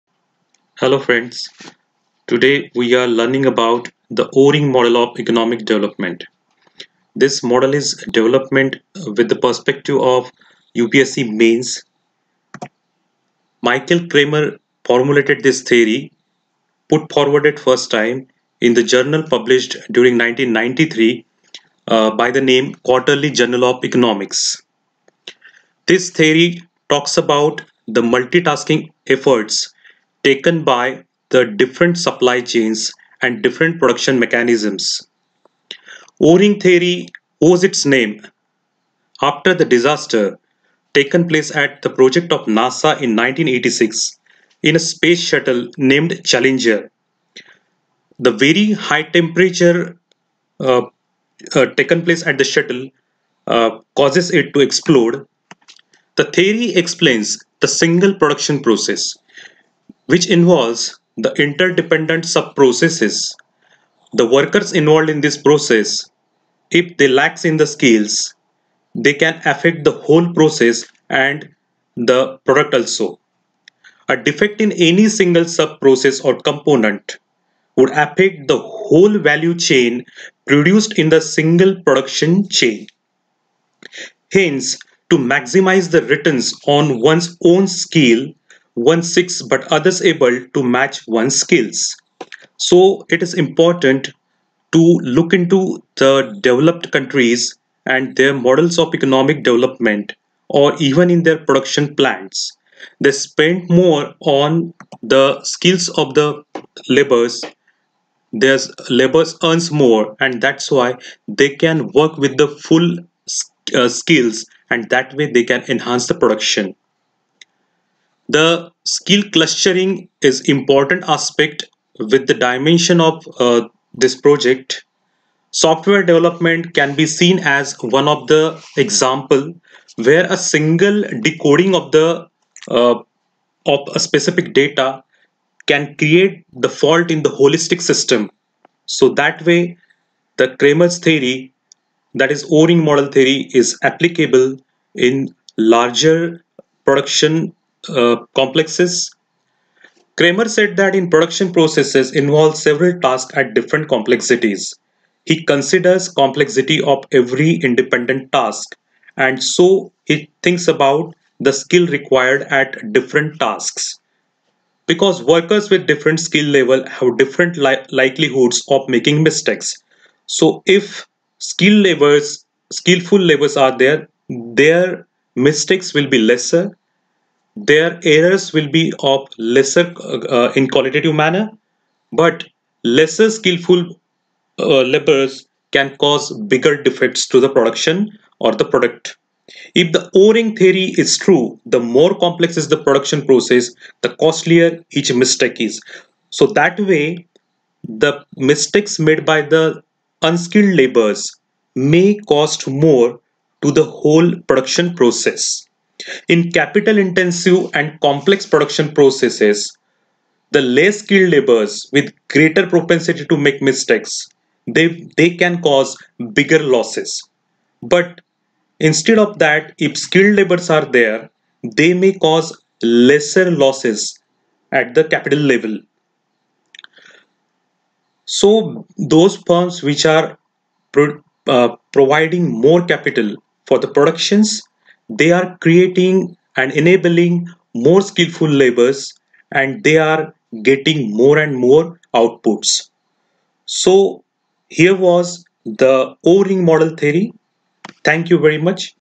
O Ring Theory of Economic Development I Michael Kremer Theory I Current Affairs for UPSC I IAS I
O Ring Theory of Economic Development. O-Ring Theory of Economic Development a model of economic development by Michael Kremer. Questions can be asked in UPSC Civil Services Preliminary, Mains exam or in Interview. Question about this topic can be asked in GS Economy, Current Affairs, Science and technology etc. Detailed article can be read on the